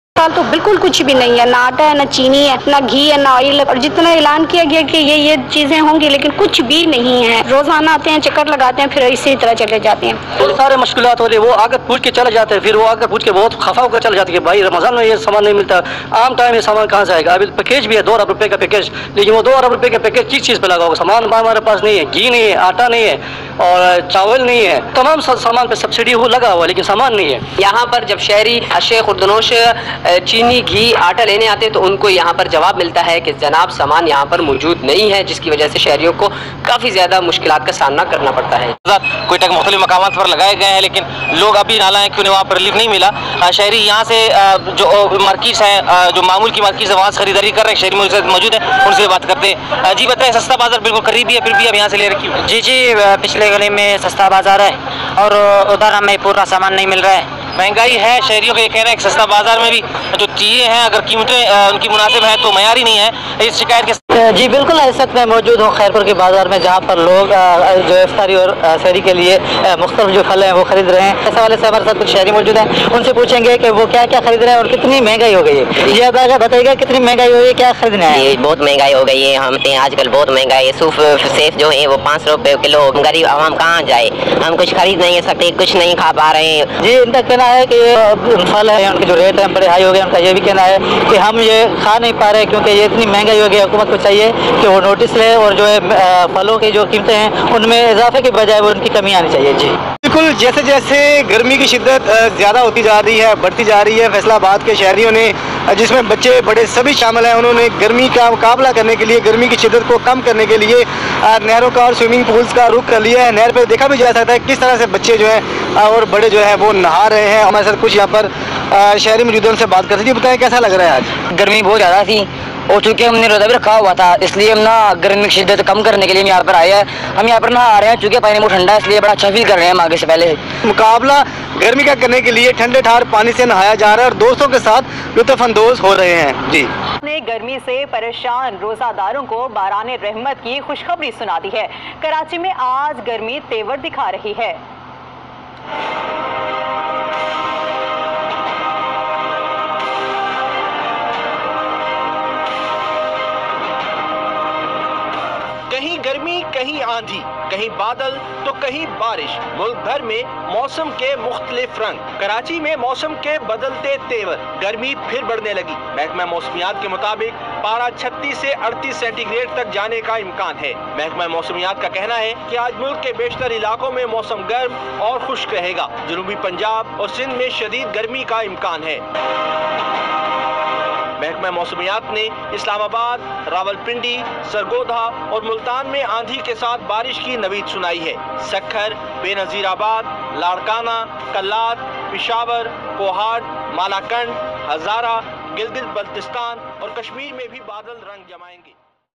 Ramzan Vs INCREASING Costliness of the country/ lastest Ary news/pakistan weather news
Latest news update Ramzan is the Islamic month for the Muslims. People are protesting against the increasing costliness of Pakistan.
Latest news
Ary news